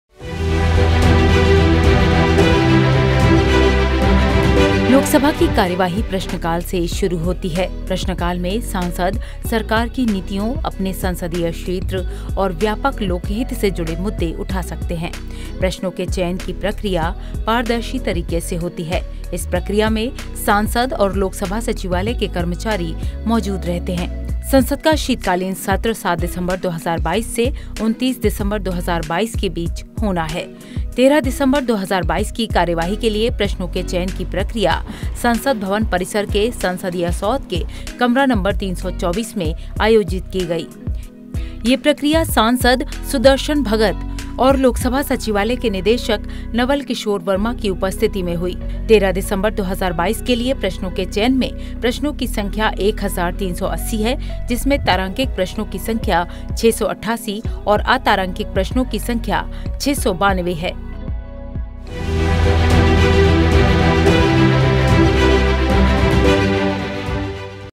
लोक सभा बैलेट प्रक्रिया प्रश्नकाल | 25 November, 2022
संसद के शीतकालीन सत्र में 13 दिसंबर 2022  को लोक सभा की बैठक के लिए प्रश्नों के चुनाव हेतु कंप्यूटरीकृत बैलेट की प्रक्रिया 25 नवंबर 2022 को संसद भवन परिसर के संसदीय सौध के कक्ष संख्या 324 में आयोजित की गई। ये प्रक्रिया सांसद सुदर्शन भगत और लोक सभा सचिवालय के निदेशक नवल किशोर वर्मा की उपस्थिति में हुई।

Producer: Archana Mishra